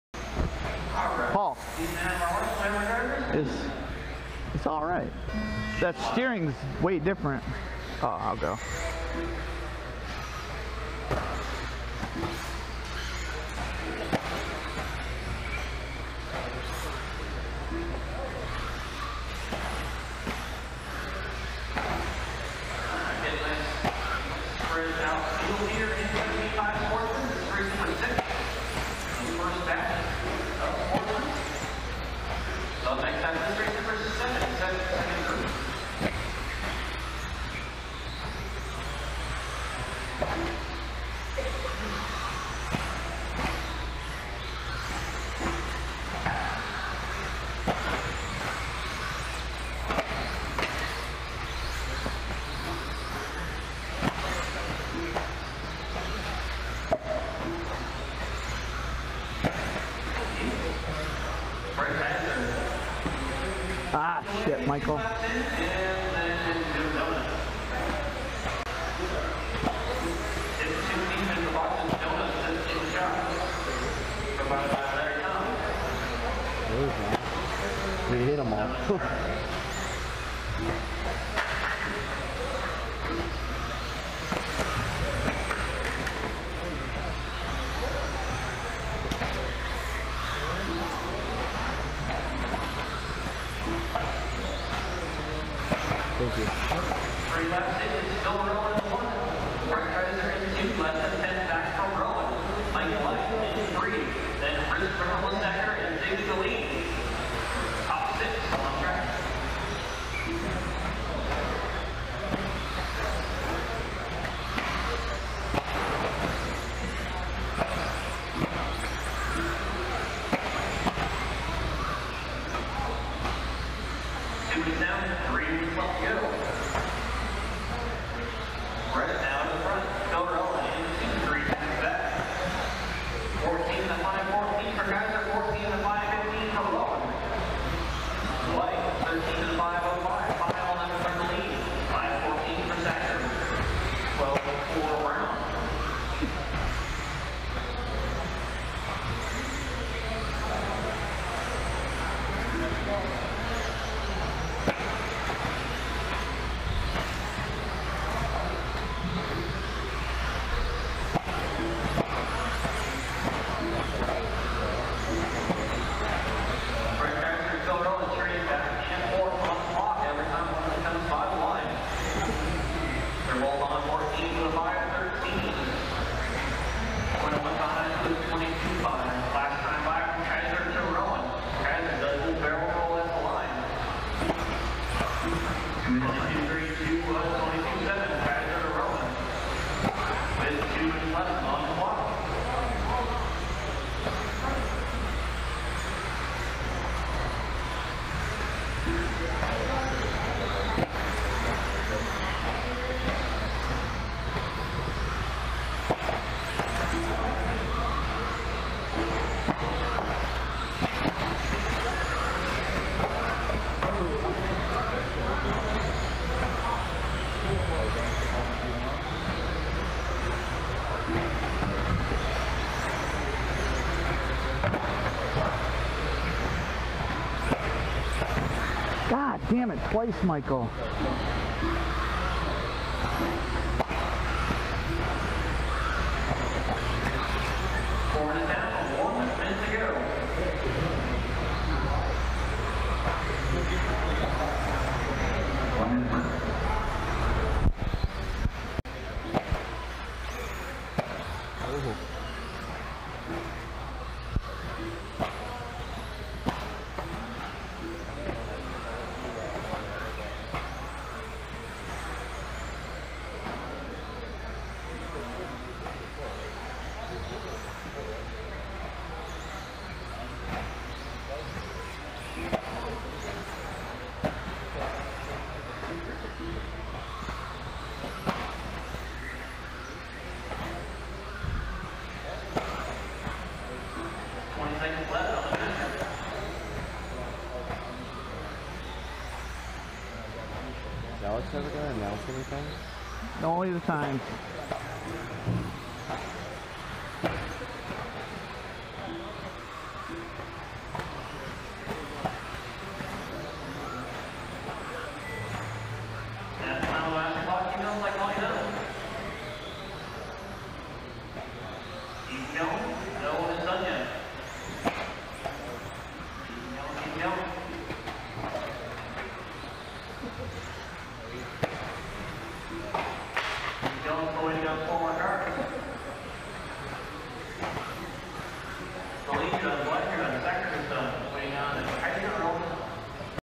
1st Qualifier 17.5 Sportsman 12-22-18 with the B6.1D
After the race on Wednesday I made a lot of changes for my next race which I thought would be next Wednesday the 26th. I stayed late and went in early Saturday to get jobs done so I could race.

I changed my diff height and inserts to +1 instead of 2. Changed the gearing a little bit. Changed the oil to 30/27.5, rear springs to yokomo greens and lengthened the front camber links. A lot of changes for a track i thought would have more grip.

I didnt push hard and caught the center pipe a couple times keeping me from hitting 14 laps.

Lap Times
# Time Pace Pos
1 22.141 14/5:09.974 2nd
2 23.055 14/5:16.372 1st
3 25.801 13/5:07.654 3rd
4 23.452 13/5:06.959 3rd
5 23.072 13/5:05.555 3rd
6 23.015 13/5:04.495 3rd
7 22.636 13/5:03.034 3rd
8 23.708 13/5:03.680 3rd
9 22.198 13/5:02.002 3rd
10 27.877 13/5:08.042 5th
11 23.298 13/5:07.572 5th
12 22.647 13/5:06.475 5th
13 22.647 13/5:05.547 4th